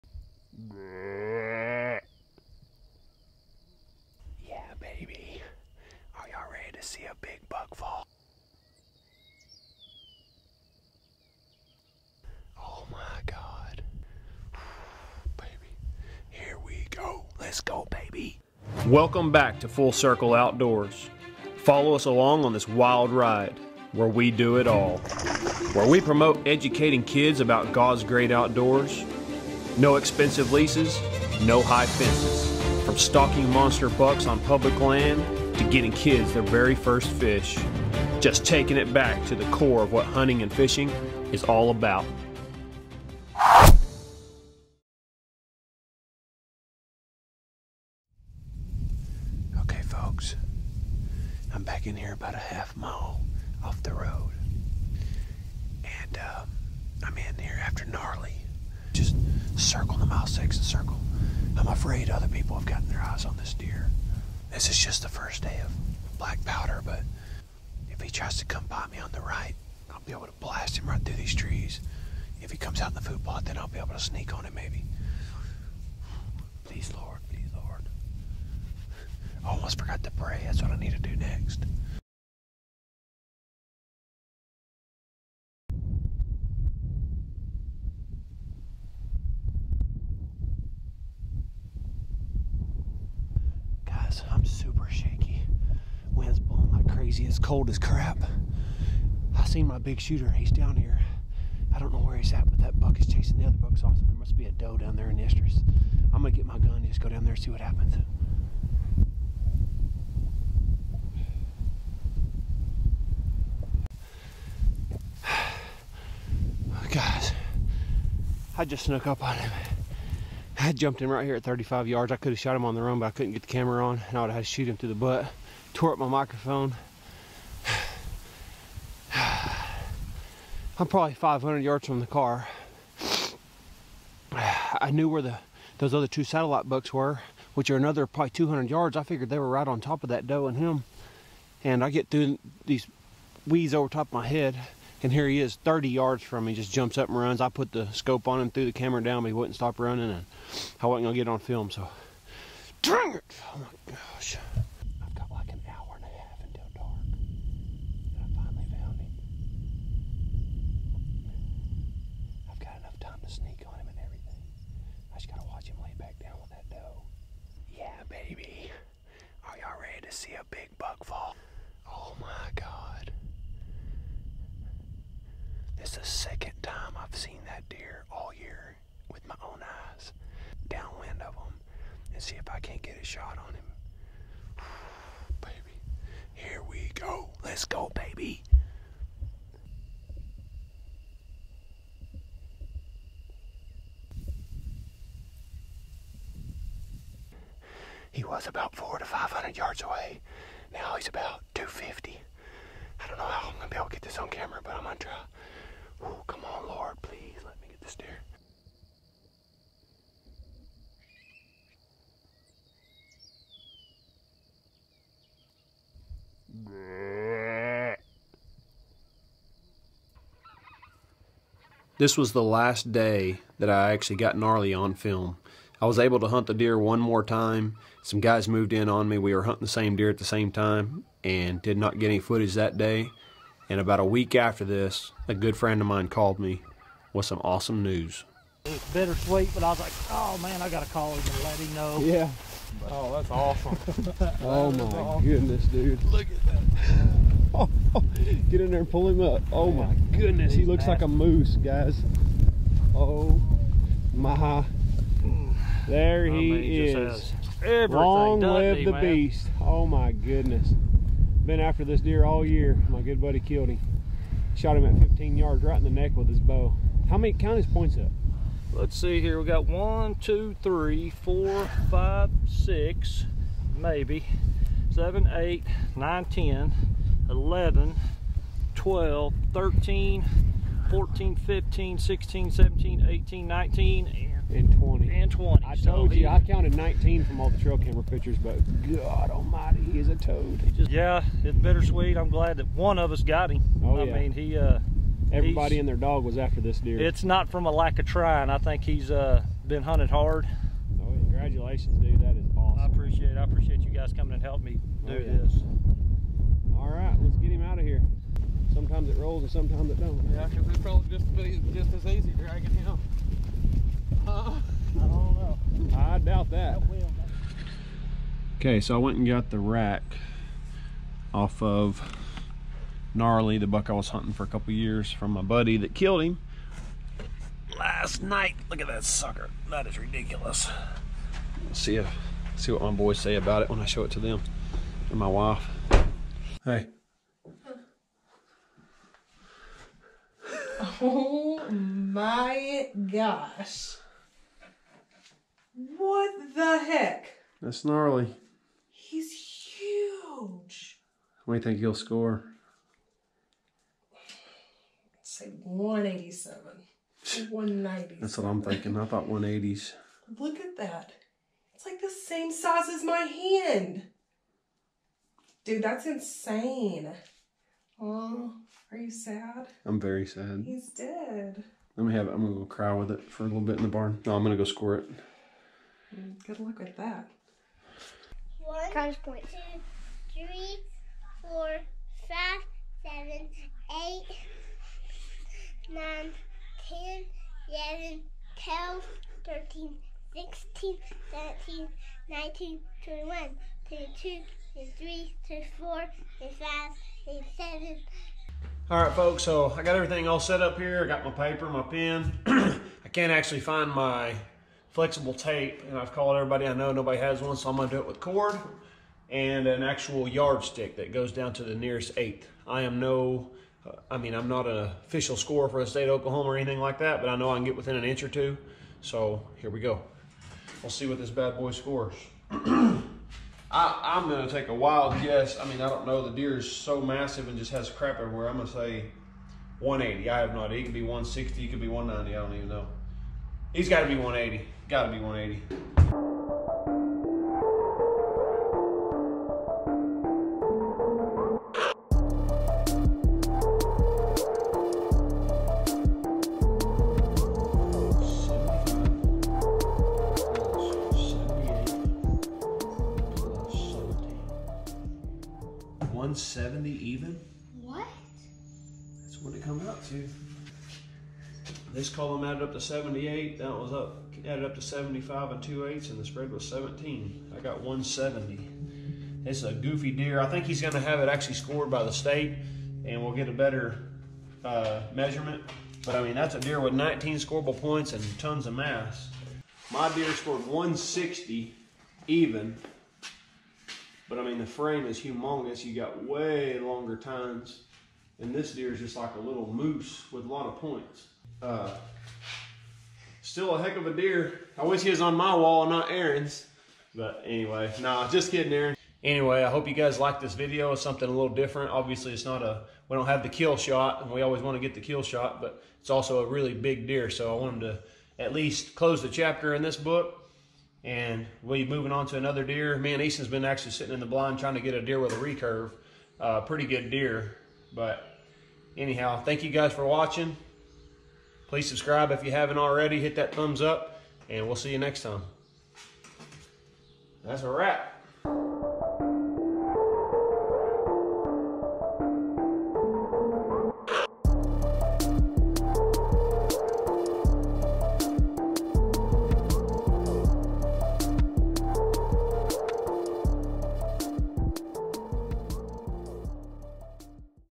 Giant Falls In Oklahoma | Short Film About Gnarly
This is a short documentary about the rise and fall of a true giant. So long Gnarly!